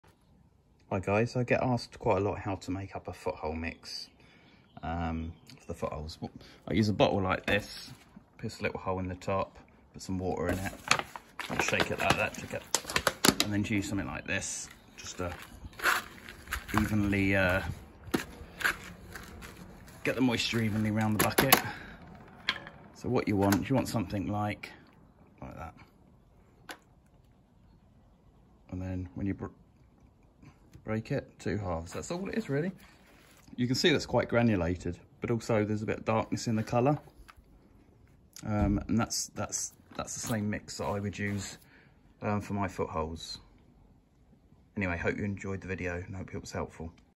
How to Make up a Foothole mix for Cricket Pitch Repairs
If you would like to know how to repair footholes between game/innings then why not also take a look at this helpful blog